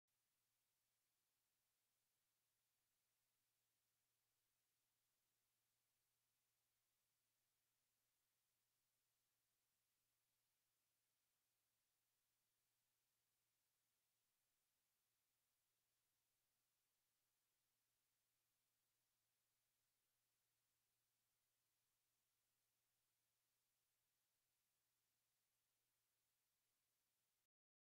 げーとほうりゅう1
ダムのゲートから放流している風景です。